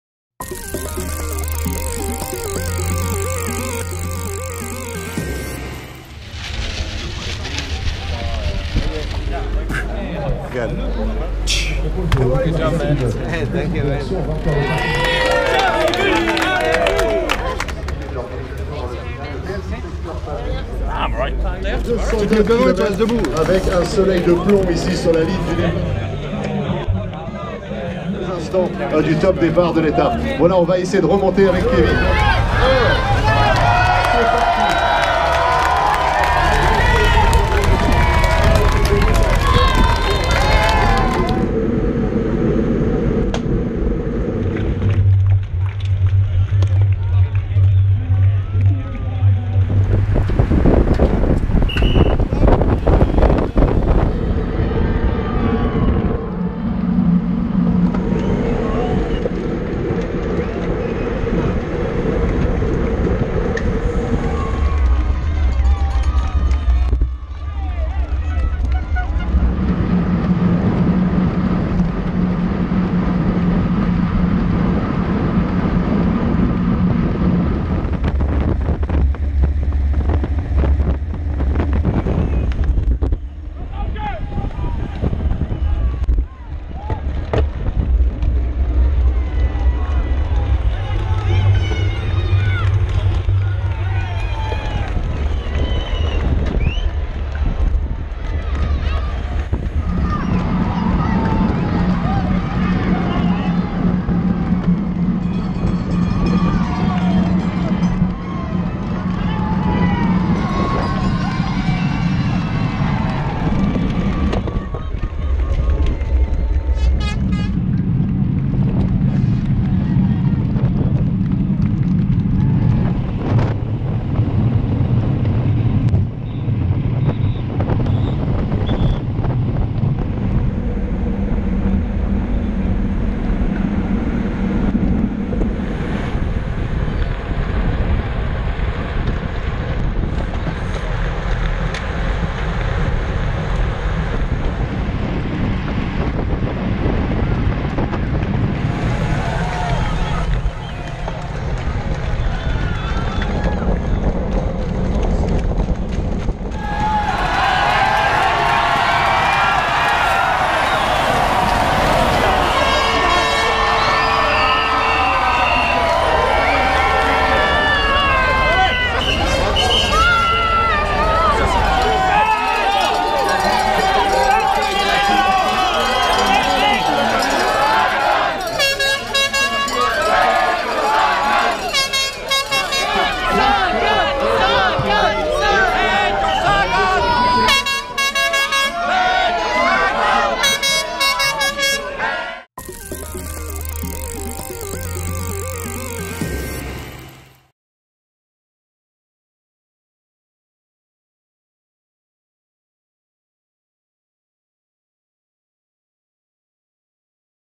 Tour de France 2016: Stage 16 on-board highlights
It was a pulsating bunch sprint finish to bring a climax to Stage 16 of the Tour de France, before a well deserved rest day for the riders. Peter Sagan won it by the narrowest of margins - watch to see an incredible finish!